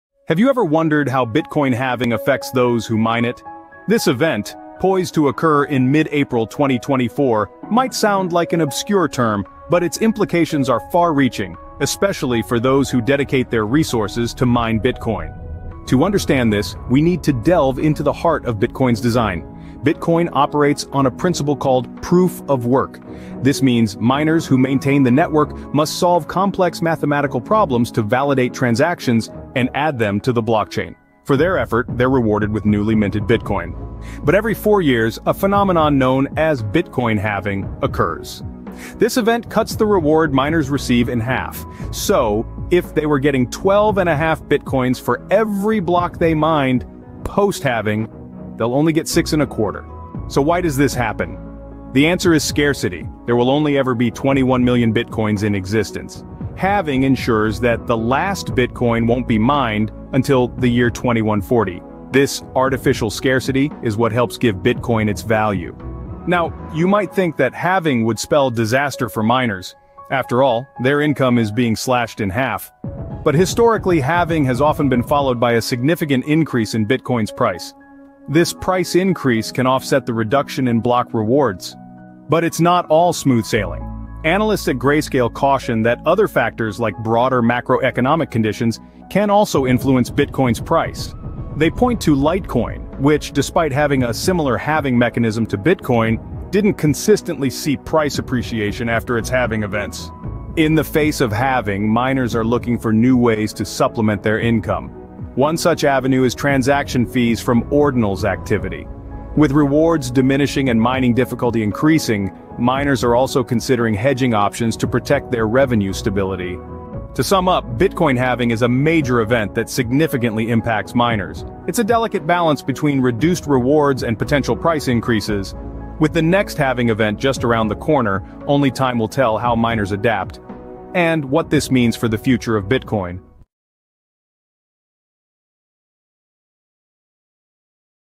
Bitcoin Halving 2024: A Revenue Challenge for Miners
In this highly anticipated video, we delve into the upcoming Bitcoin halving event in 2024 and its potential impact on miners' revenue. Join us as we break down the latest news and analysis surrounding this pivotal event in the cryptocurrency space.

Bitcoin halving is a fundamental event that occurs approximately every four years, and it has significant implications for Bitcoin miners. By reducing the rate at which new Bitcoins are generated, the halving event brings both challenges and opportunities for miners.

Throughout this video, we'll explore the factors contributing to miners' revenue and discuss how the upcoming halving event may affect their profitability. Our expert analysis will shed light on the potential risks and rewards associated with Bitcoin mining post-2024.

Whether you're a seasoned miner or just getting started, this video is packed with invaluable insights to help you navigate the ever-evolving world of cryptocurrency. Don't miss out on this informative discussion!

OUTLINE:
00:00 Introduction to Bitcoin Halving
00:15 Understanding Bitcoin's Design
00:33 The Concept of Bitcoin Halving
00:50 The Reason Behind Bitcoin Halving
01:05 Impact on Miners
01:21 Other Factors Influencing Bitcoin's Price
01:40 Miners' Strategies
01:56 Conclusion